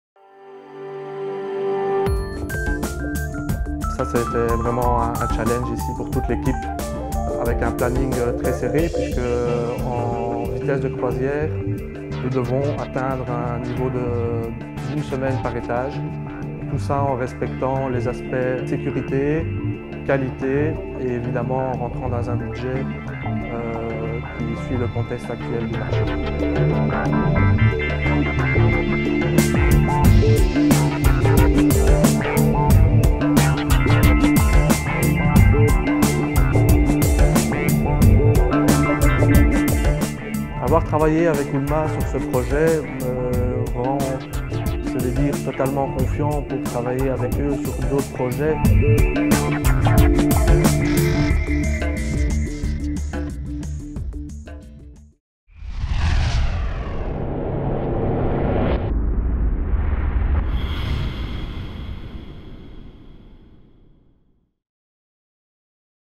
Chef de Chantier, Belgique - Why ULMA [fr]
Why ULMA: Philippe Haubert - Chef de Chantier:

La tour upsite sera la plus haute tour résidentielle  de la Belgique et une des plus haute de l’Europe. Ce type de construction n’était plus fait depuis 20 ans dans notre pays. De plus le planning est très serré puisque en phase de croisière il faut atteindre une étage par semaine tout cela en respectant les critères de qualité et de sécurité adapter à ce type de projet.

ULMA nous a apporté des solutions techniques, spécifiques adapté à tous les besoins du projet. Ulma nous a également apporté un support technique depuis le début du projet et à chaque étape suivante.Nous avons également eu un package complet incluant tous les services. ULMA a pu nous garantir de fournir tout le matériel à temps et nous a apporté des solutions pour pré monter  tous les consoles et voiles dans un délai rapide. Ce qui rend compliqué le design de cet auto grimpant.

Je dirai, de travailler avec ULMA c’est travailler en confiance sur des projets innovants et en toute flexibilité.

PROJET:

Le projet de l’auto grimpant sur la tour Upsite n’est pas un auto grimpant très simple parce que nous avons en effet deux noyaux a appliqué l’un dans l’autre avec des ailles qui partent de chaque côté.

ULMAa apporté des solutions techniques et a désigné  tous les consoles en fonction de cette géométrie spéciale.

Plus d'informations sur le projet de la Tour Upsite de Belgique

Why ULMA: Philippe Haubert - Jobsite Manager

This project has been a challenge for the entire team. We worked under a very demanding schedule, completing one storey per week. Nevertheless we were able to uphold the highest standards of both safety and quality, while of course maintaining a competitive budget.

After working with ULMA on this project, we trust in them completely to work on other complex projects.

PROJECT:

Its 142 m and 42 floors make the Upsite Tower the tallest residential building in Belgium and the first skyscraper built in Brussels in the past 30 years. Apart from flats, it will also accommodate offices, car park and garages, restaurants, shops, etc.  The floor height ranges between 3.1 and 3.75 m, and the slab thickness between 0.24 and 0.28 m. The core of the building was built with the ATR self-climbing system and narrow platforms for inside shafts with vertical beam-based formwork which provided the required versatility.

The working cycle achieved with the self-climbing system was 7 days. Platforms with a maximum width of 12 m and narrow platforms and solutions for inside shafts were applied. Aluminium panels were used as slab formwork. Two sets of props increased efficiency by being able to recover most of the formwork before hardening of the concrete was completed.

The HWS Hydraulic Windshield System increased workers' safety at the slab edge with large hydraulically lifted screens covering the entire perimeter of the building.